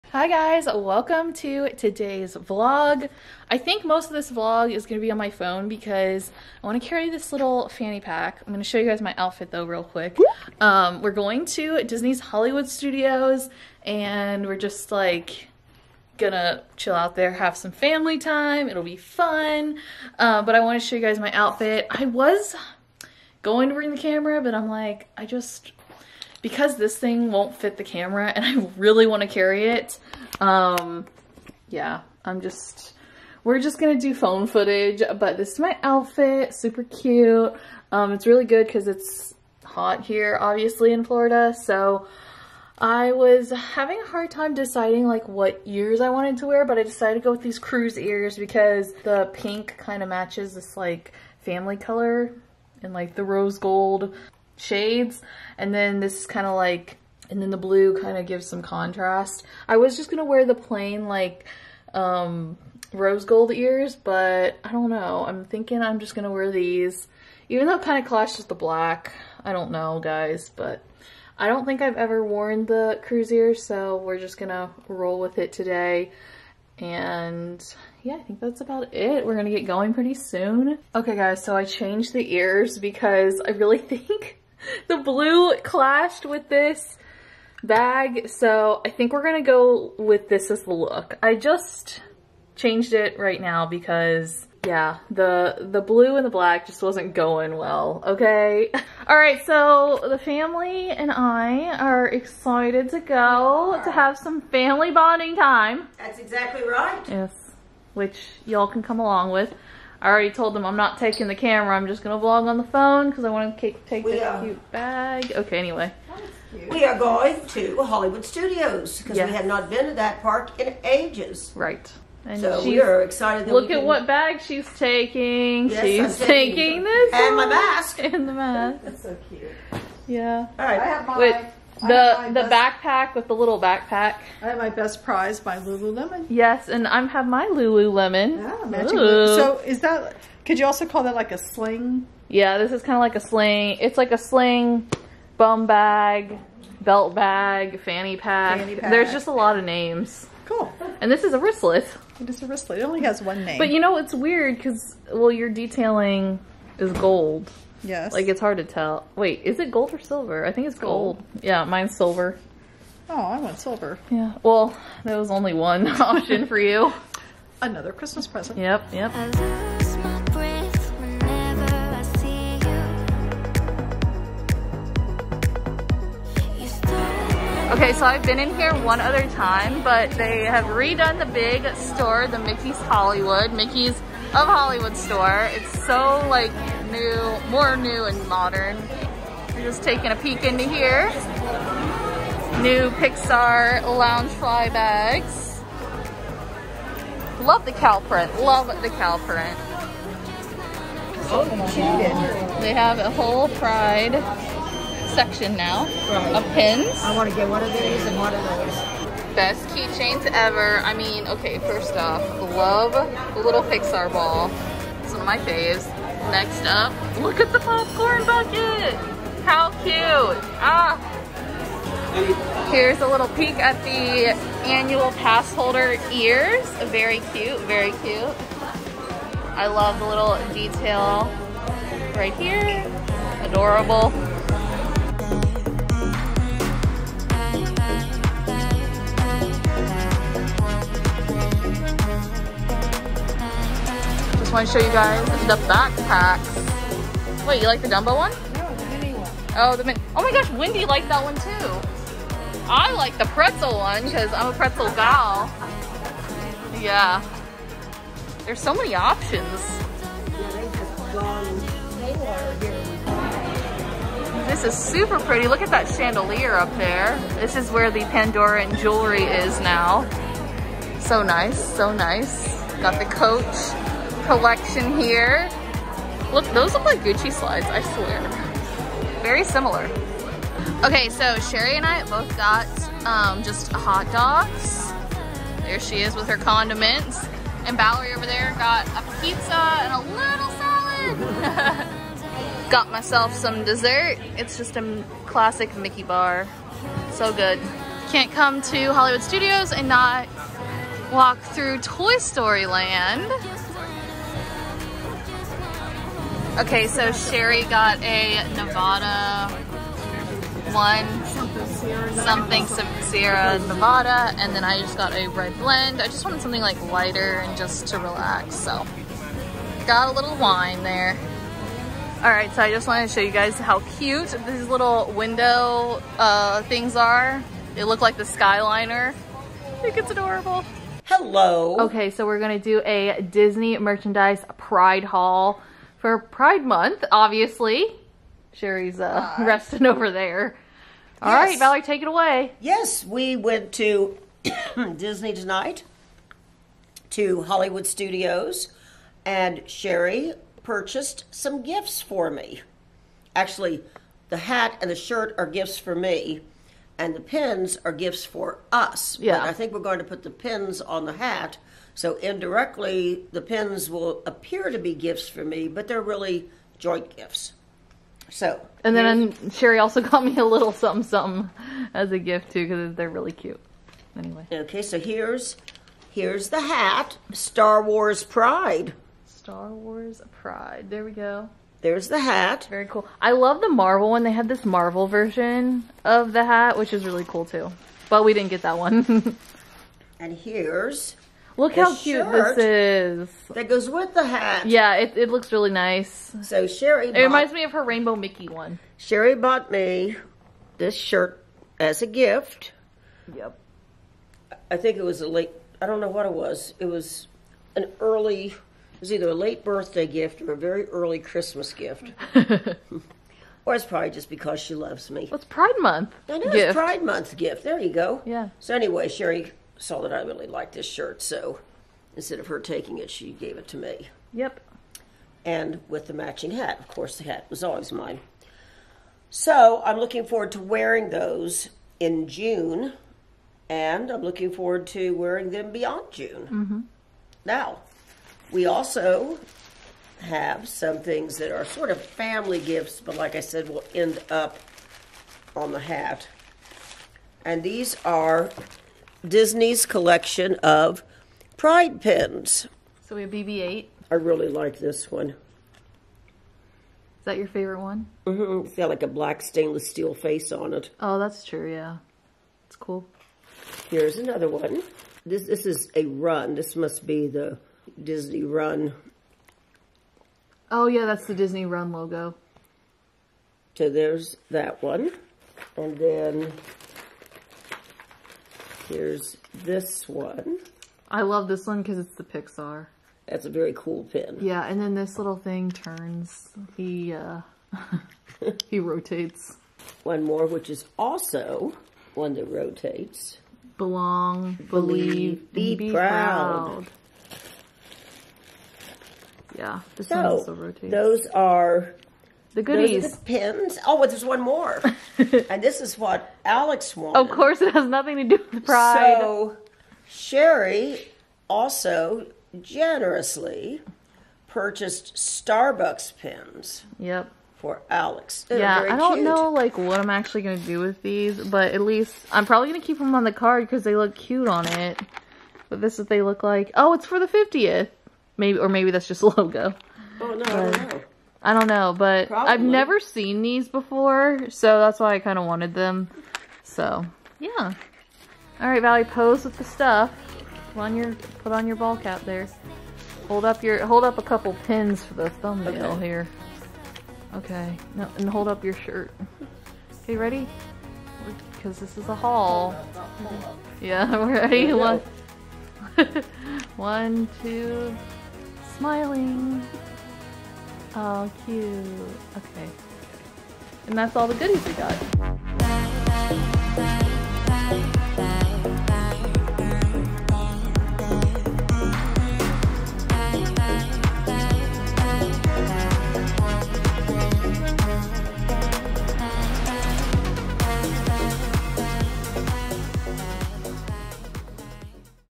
Disney PRIDE Collection Haul! 2022
✩ Disney PRIDE Collection Haul! 2022 In this vlog my moms and I go to Disney's Hollywood Studios. We check out and purchase all the Disney PRIDE Collection merchandise. We also show you all the Disney PRIDE Merchandise we picked up. All around it was a fun family day at Walt Disney World's Hollywood Studios.